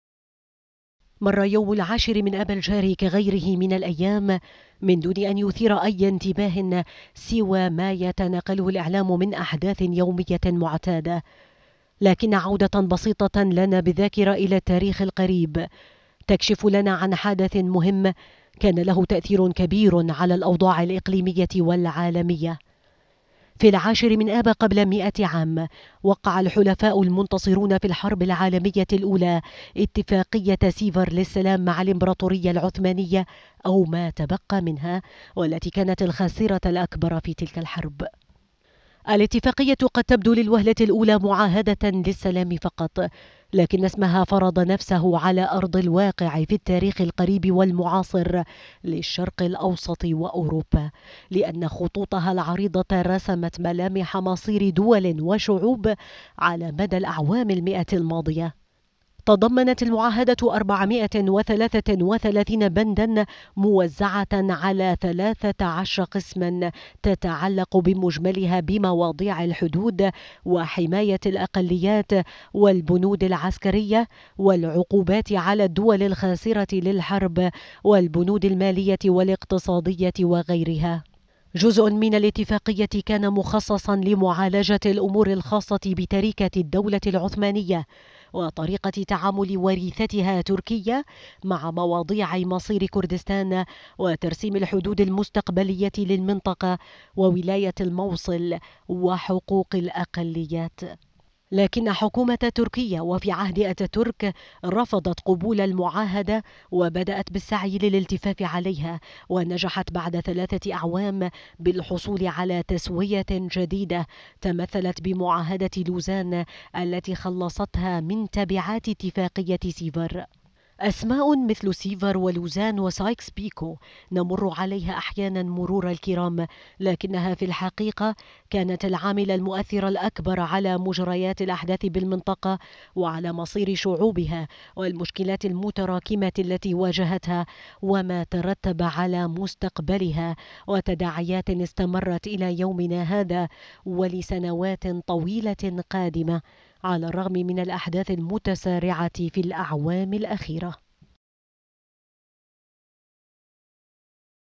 معاهدة سيفر .. هل هي سبب الوضع الراهن في المنطقة؟ | تقرير
لم يثر بالأمس مرور مئة عام على توقيع معاهدة سيفر، الكثير من الانتباه او الاهتمام، بسبب انشغال العالم بالأحداث الراهنة والساخنة، لكن العودة لصفحات التأريخ ومطالعتها بتمعن، ستظهر لنا ان معاهدات مثل سيفر ولوزان وسايكس بيكو كانت هي الأشد تأثيرا، في تحديد مصير دول وشعوب على مدى العقود الماضية، وربما لسنوات أخرى مقبلة.